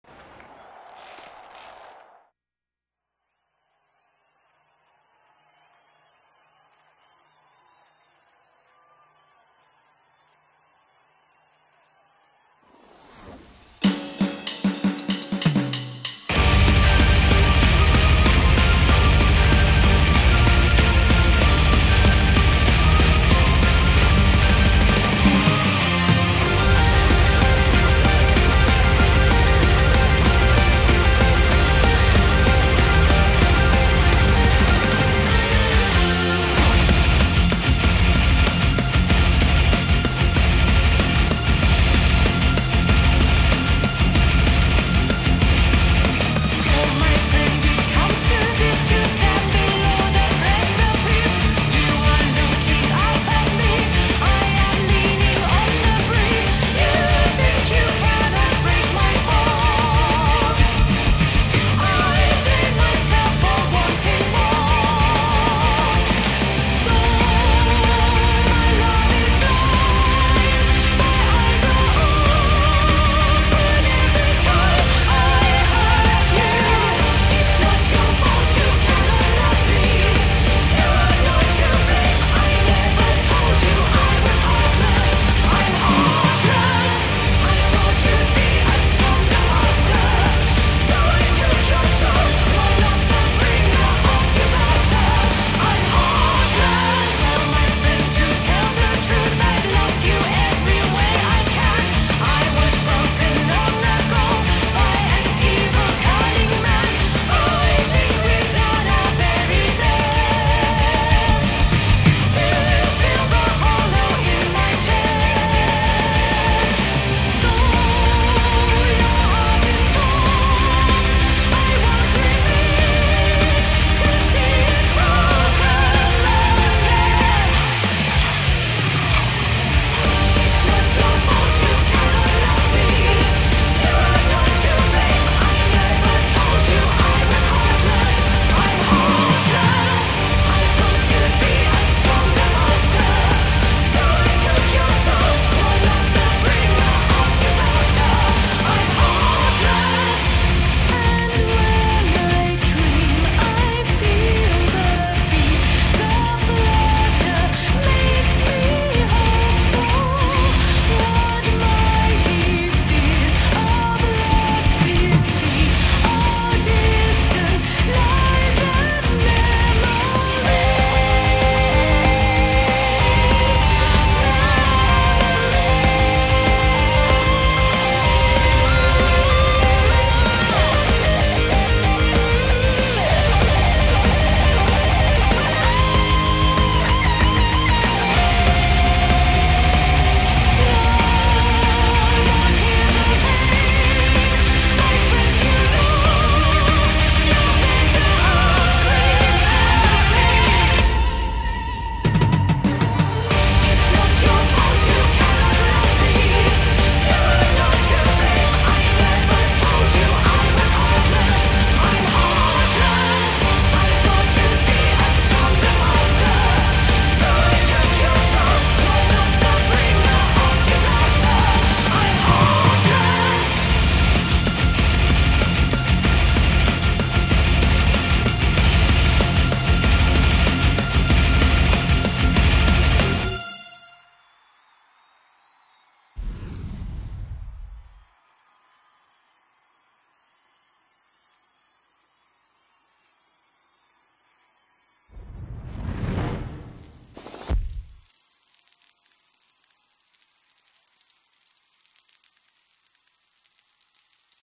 Ok, is this better :)
There, it's HD. Pls stop complaining now. Also this song is fucking sexy. Good warmup for your leg lol. Guitar is a good warmup for your arm too. This song can give you a full body workout :D